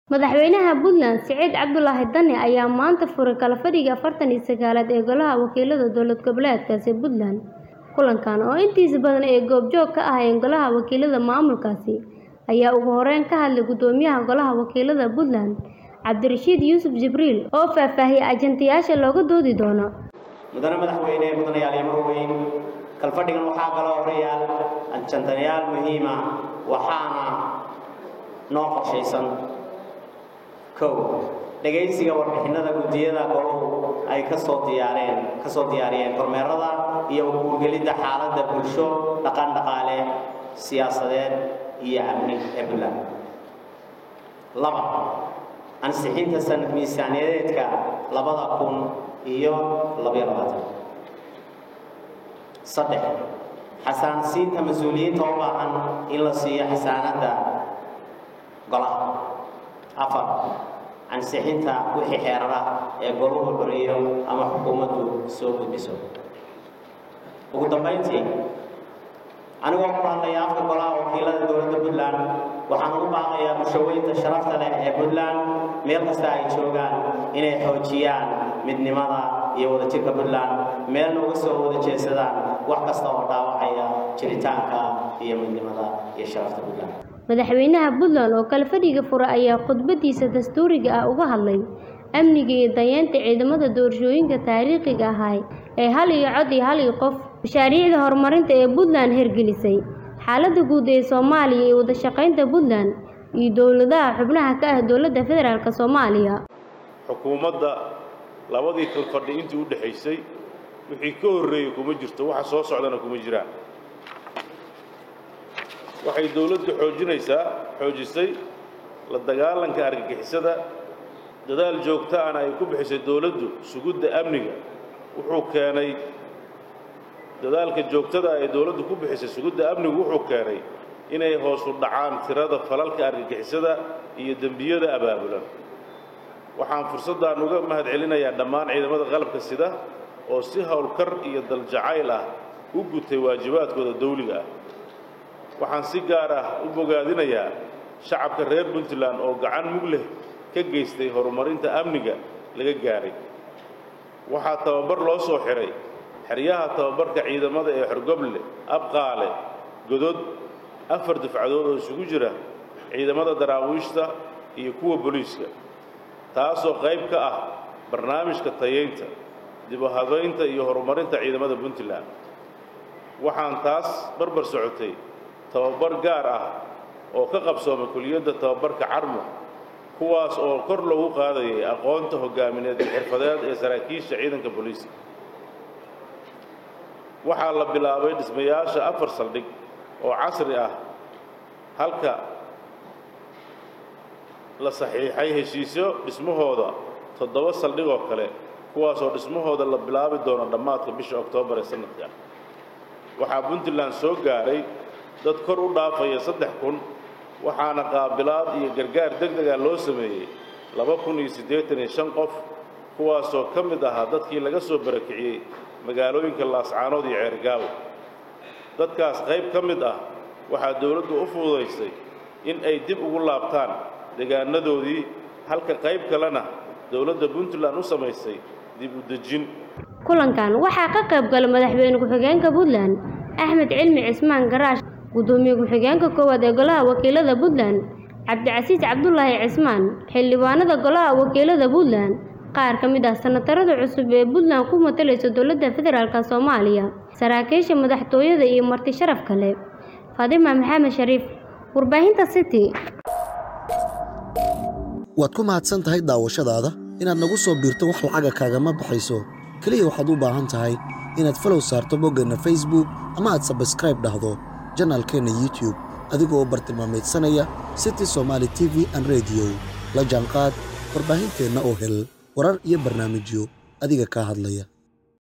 Kalfadhiga 49aad ee Puntland oo laga Furay Magaalada Garoowe ee Caasimada Maamulka Puntland
Kalfadhiga 49aad ee Puntland oo laga Furay Magaalada Garoowe ee Caasimada Maamulka Puntland Fadumo Maxamed ayaa Kawaramaysa